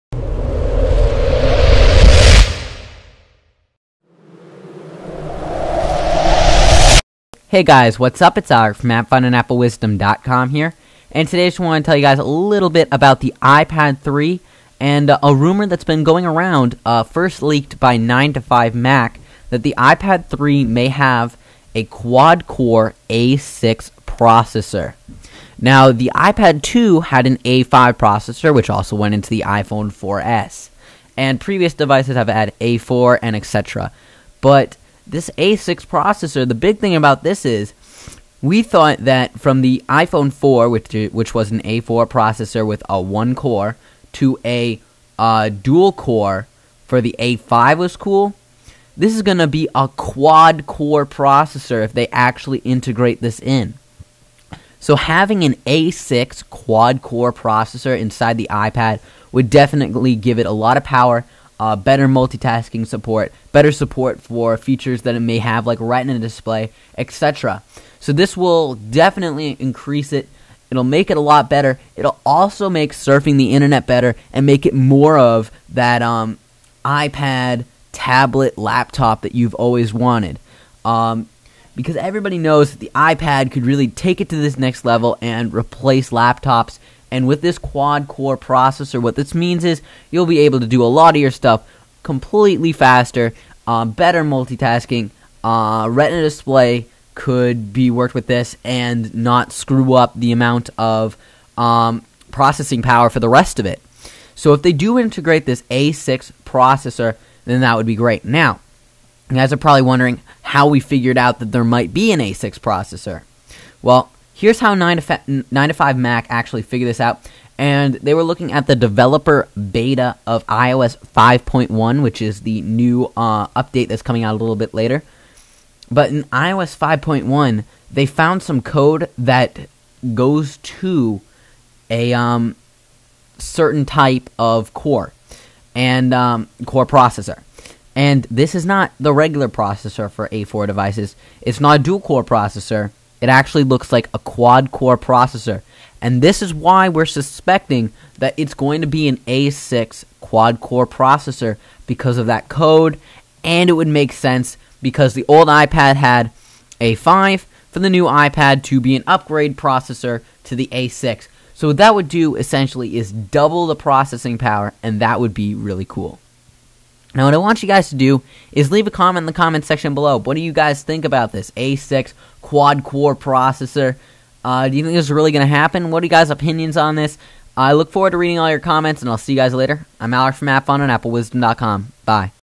A6 Quad Core Processor in iPad 3 - LEAKED
As reported by multiple sites such as 9to5Mac, there is a big leak/rumor out there about the iPad 3 containing an A6 Quad Core Processor inside of it. Watch the video for more details.

The image in this video is from 9to5Mac used for news and reporting purposes.